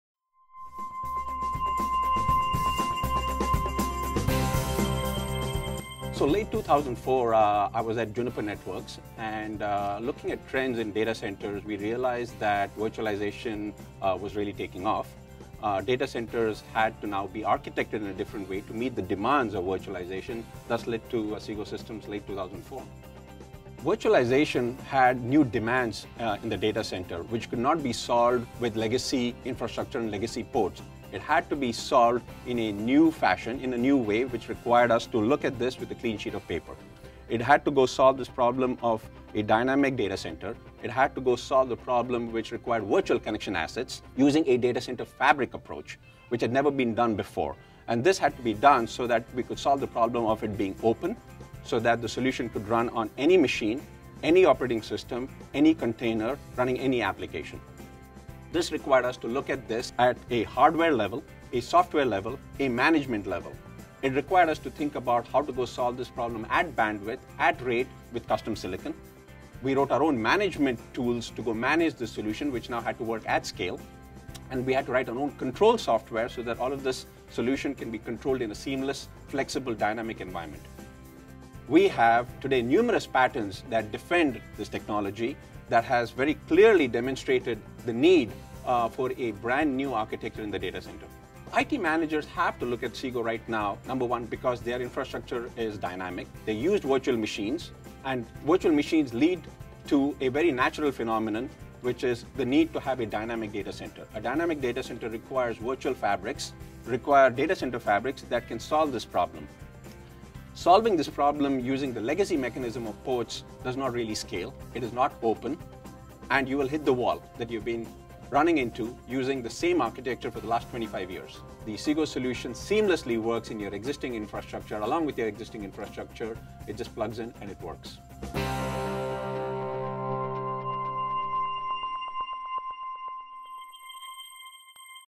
Xsigo Data Center Fabric Technology
Xsigo Founder Ashok Krishnamurthi discusses Xsigo Data Center Fabric and its technology development.

In this 2 minute video, learn how the data center fabric forms the foundation of the next gen virtualized data center. When used with server virtualization, the data center  fabric architecture completes the vision of the fully virtualized data center, allowing far more agility and performance than is possible with traditional server connectivity.